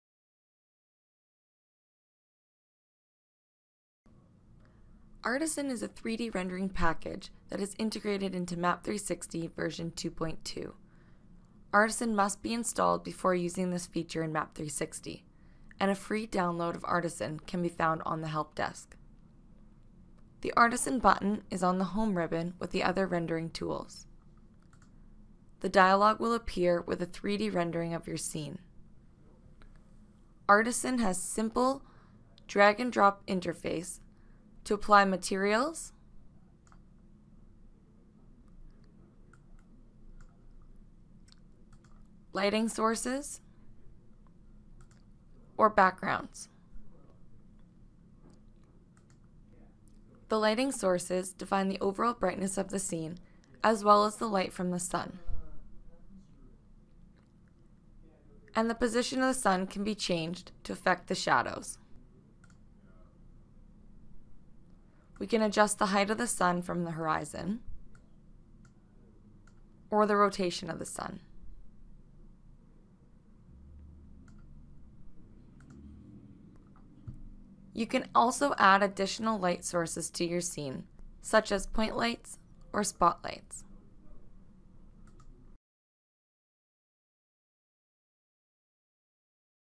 Map360 2.2 - Artisan Rendering
The intuitive 3D rendering system, Artisan, has been integrated into Map360!  The drag-and-drop interface lets you easily create a photorealistic image of your drawing by adding materials, backgrounds, lighting and shadows.